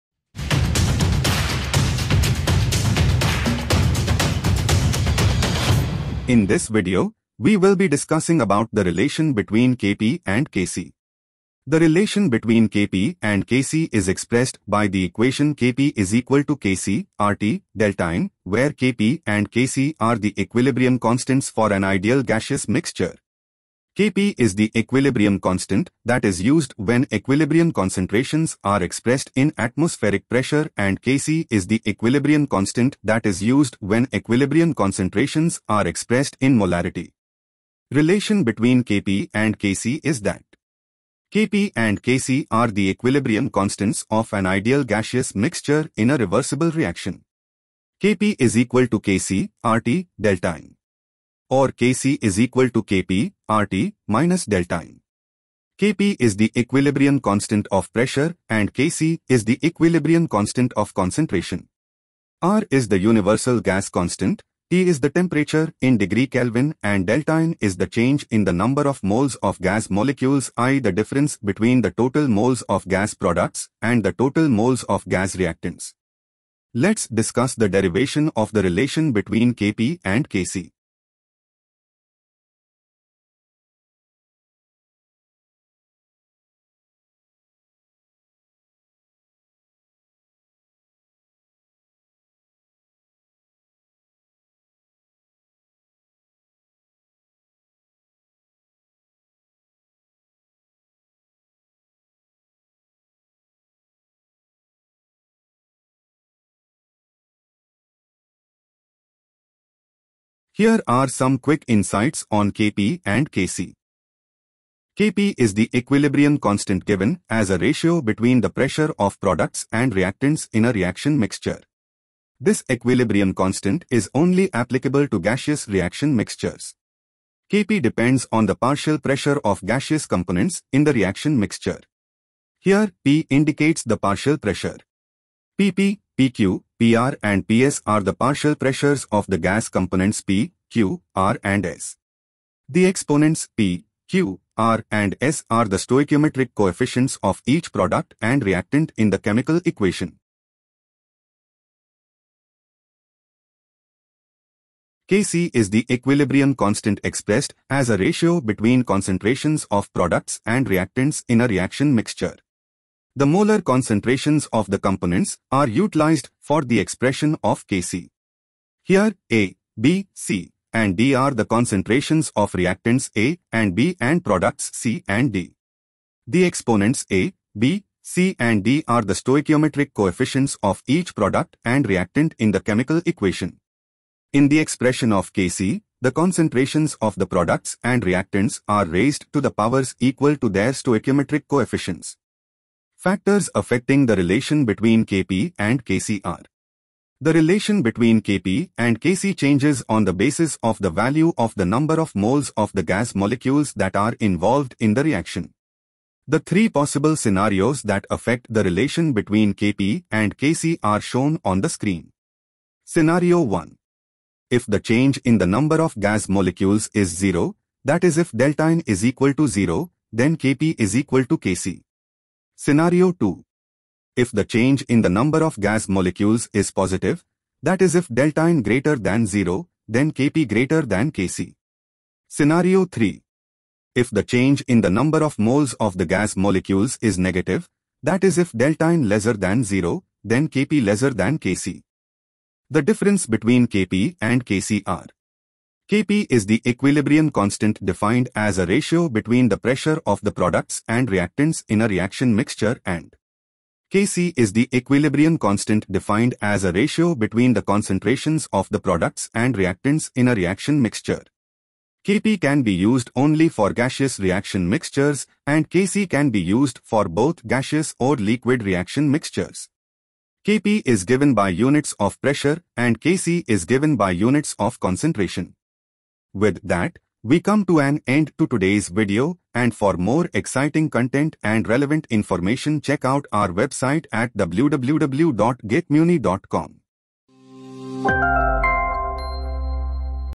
Relation Between Kp and Kc | Kp & Kc Formula| Derivation and FAQs | GetMyUni
In this video, we will be discussing the relation between Kp and Kc

The relationship between Kp and Kc is expressed by the Kp = Kc(RT)Δn, where Kp and Kc are the equilibrium constants for an ideal gaseous mixture. Kp is the equilibrium constant that is used when equilibrium concentrations are expressed in atmospheric pressure and Kc is the equilibrium constant that is used when equilibrium concentrations are expressed in molarity.

Watch this video to know more about Kp & Kc,  Derivation of Kp & Kc, the Difference between Kp and Kc and their relations.

Time Codes
00:00 - Introduction
00:06  - GMU Intro
00:10 -  Relation Between Kp & Kc
04:40 - Conclusion

Everything you need to know about Courses